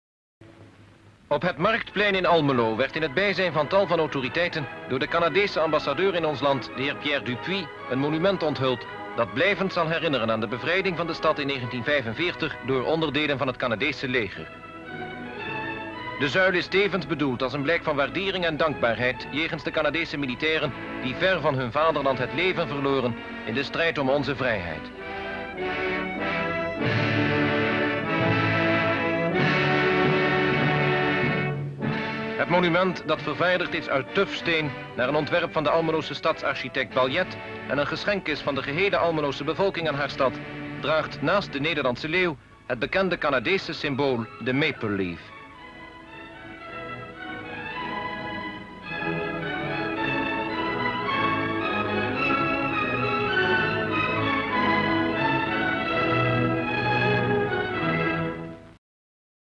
Unveiling of the liberation monument (Week number 51-54) 🔴
Description (more): The ambassador of Canada, Pierre Dupuy, unveils a monument on the market square in Almelo, which commemorates the liberation of the city by the Canadians in 1945. SHOTS: - A monument stands in the middle of the square, the plinth covered by a cloth; - soldiers are lined up; - the ambassador moves forward, followed by other notables, and pulls the cloth from the plinth and the text on the plinth becomes visible; - close up of the monument; - the notables hold a moment of silence; - scouts hoist the flag; - soldiers march past the statue (not really visible); 00:36 designer of the statue, mister Baljet.